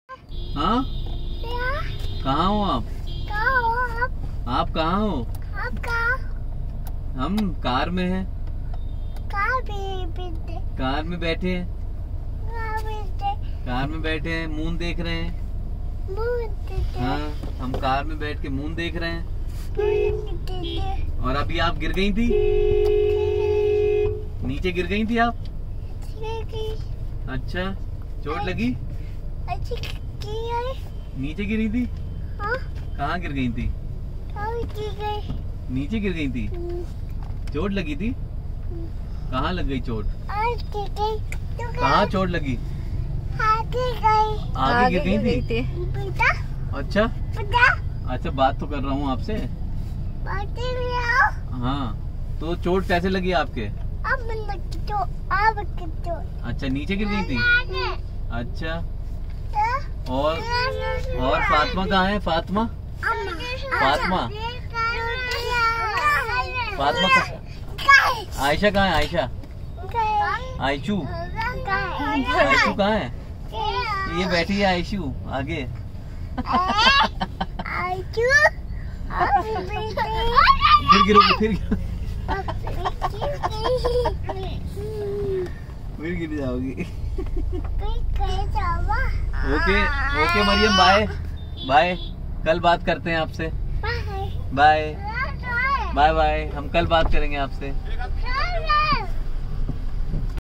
Maryam ka phone pe baat krne ka natak | Triplet’s betiyon ki masti
Hello doston, aaj main aapko apni triplet’s betiyon ki ek bahut hi cute aur funny video dikhane wali hoon. Is video mein aap dekhenge ki meri beti Maryam kaise phone pe baat krne ka natak karti hai aur uski behenein kaise uska mazak udati hain. Aapko ye video bahut pasand aayegi.
Aur comment section mein bataiye ki aapko Maryam ka natak kaisa laga.